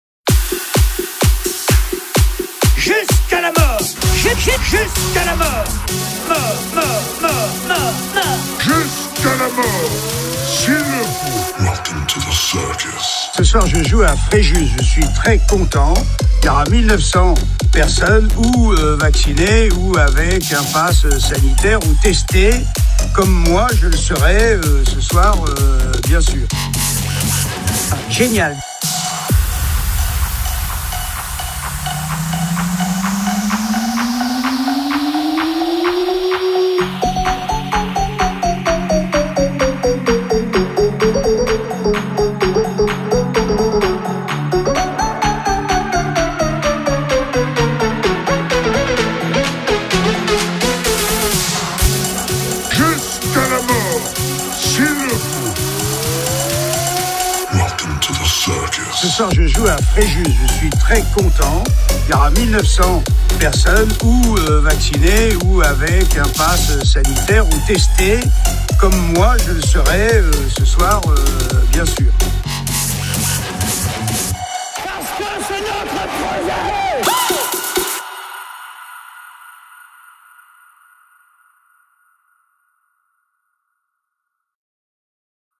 JEAN MARIE BIGARD JUSQU'A LA MORT DU DERNIER MOMENT :)
Encore une nouvelle fois les gilets jaunes sont incapables d'appliquer leurs rêgles à eux mêmes